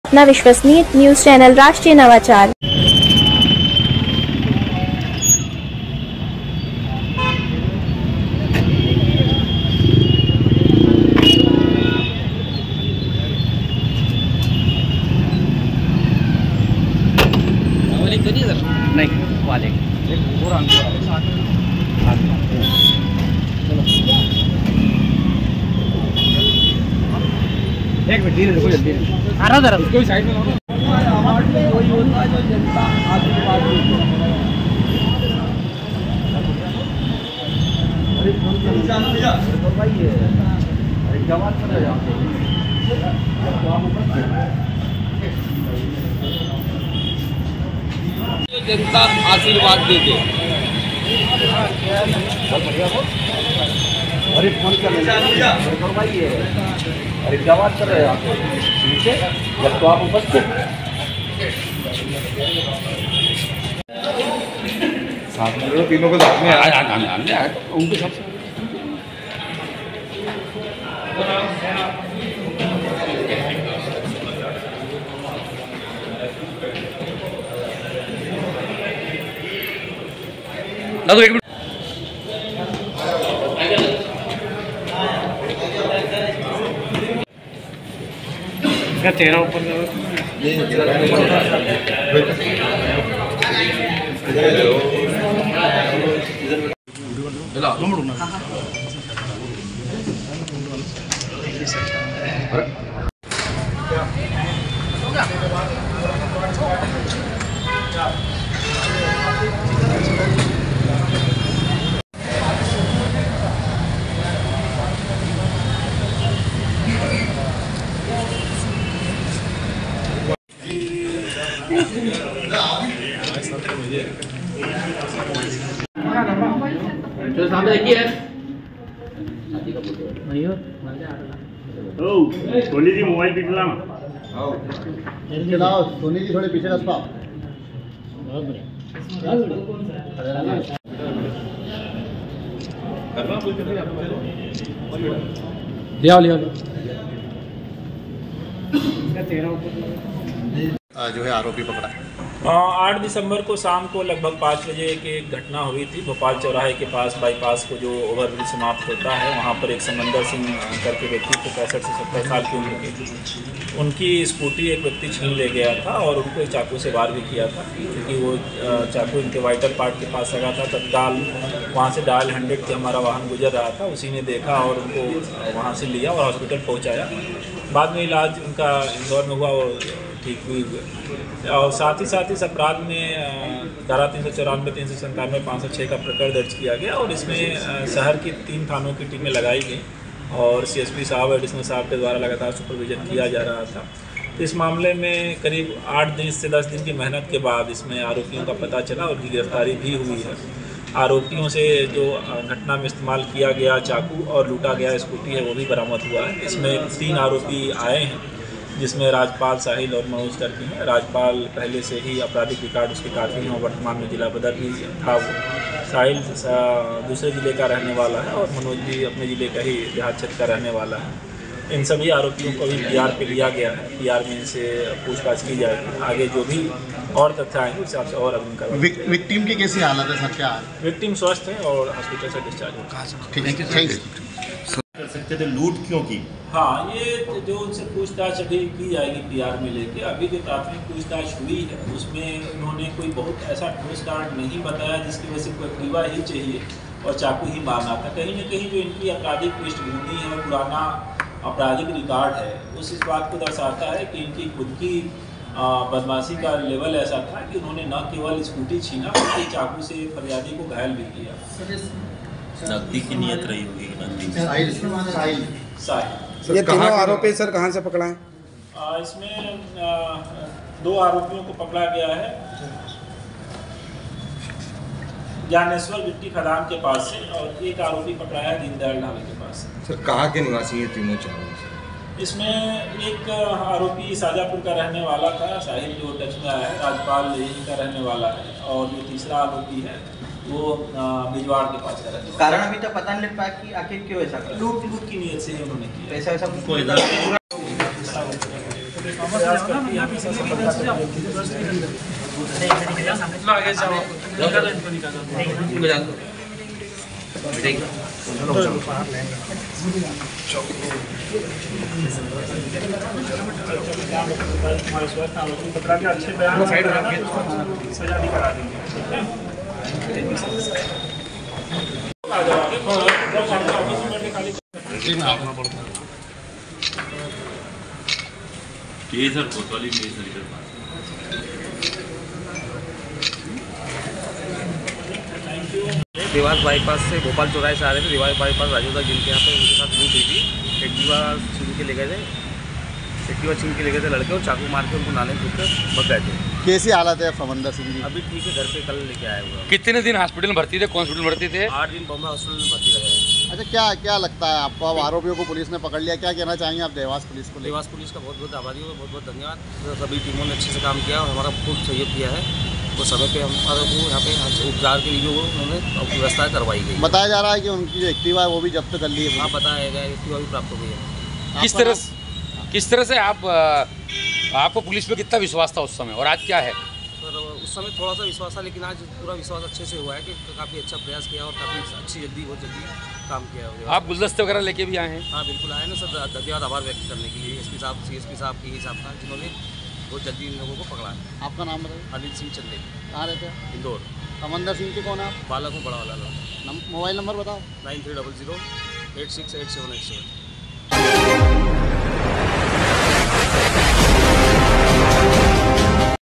हत्यारा, पिवर अपराधी..प्रोफाइल पर लिख रखे हैं आरोपी ने।राजपाल चौहान, साहिल खां व मनोज अलावे गिरफ्तार
👉 *DRN : हत्यारा, पिवर अपराधी जैसे शब्द फेसबुक प्रोफाइल पर लिख रखे हैं लूट के आरोपी ने। राजपाल चौहान, साहिल खां और मनोज अलावे ने चाकूबाजी के बाद लूट की थी बुजुर्ग के साथ। देखिए क्या कहा पुलिस अधीक्षक संपत उपाध्याय और फरियादी के पुत्र ने।*
👉 *पढ़ें : कई अपराध दर्ज हैं विभिन्न थानों में बदमाशों के खिलाफ। पढ़ें पूरी खबर पुलिस कैसे पहुंची बदमाशों तक।*

👉 *rashtriyanavachar राष्ट्रीय_नवाचार ऑफिशियल चैनल से जुड़ने के लिए लिंक पर क्लिक करें*